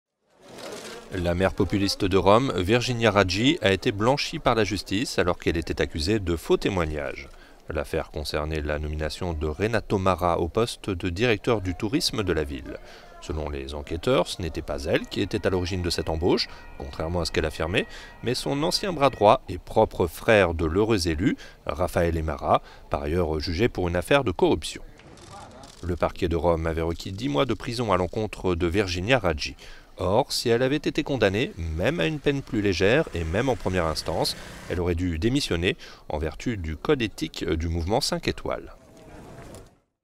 La maire de Rome innocentée par la justice
Pas de peine prononcée à l'encontre de Virginia Raggi qui était jugée pour faux témoignage.…